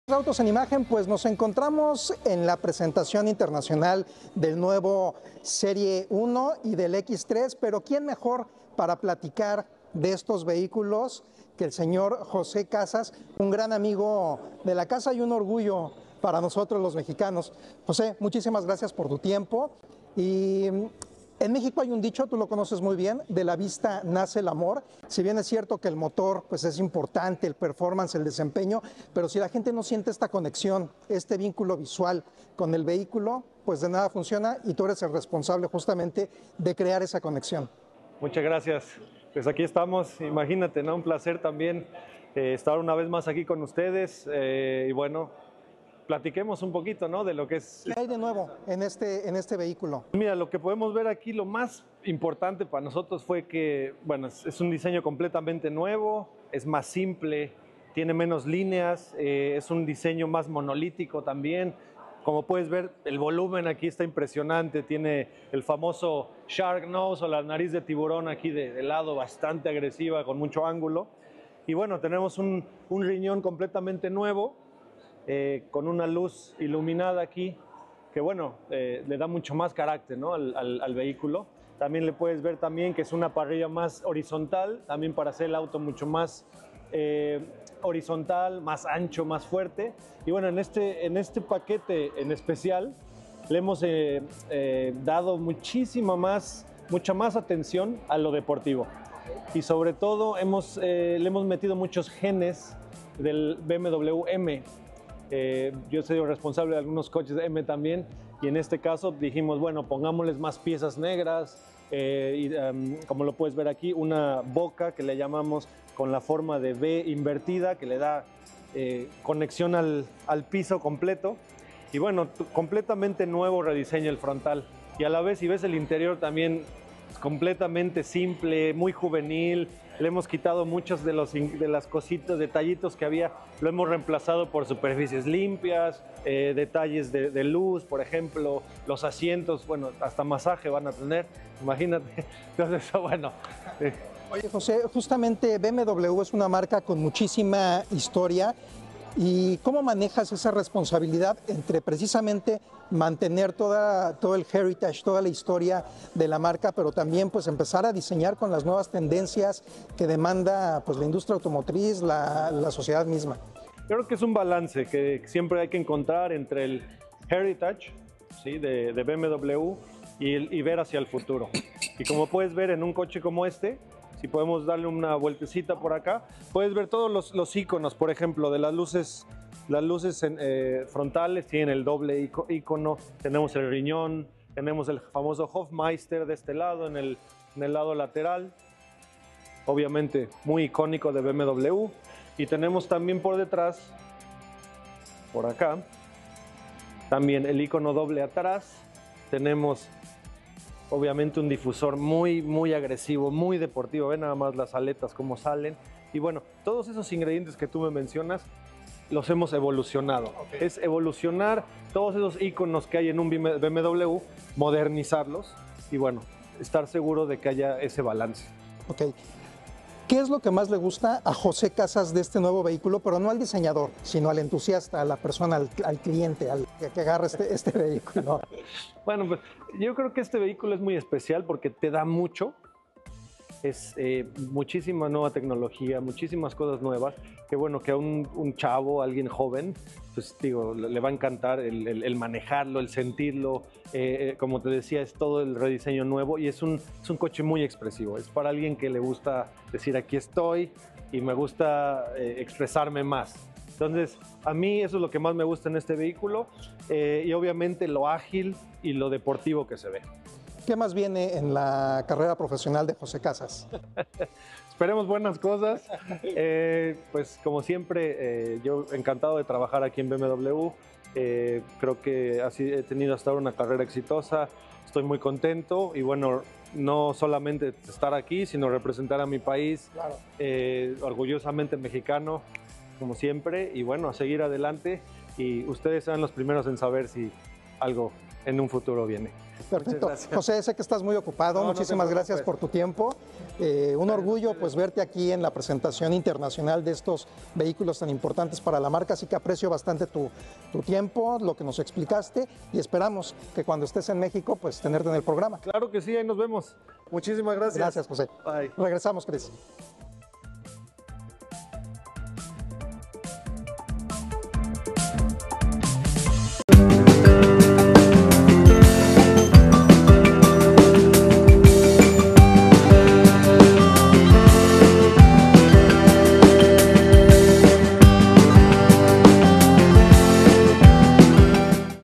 BMW presenta los nuevos Serie 1 y X3 con más tecnología y eficiencia | Autos en Imagen
En una reciente entrevista, José Casas, diseñador de BMW, nos habla sobre la presentación de los nuevos modelos Serie 1 y X3. Destacó las mejoras en tecnología, diseño más dinámico y mayor eficiencia. Además, resaltó el enfoque de la marca en la innovación y la sostenibilidad en estos vehículos, que buscan mantener el liderazgo en el mercado premium.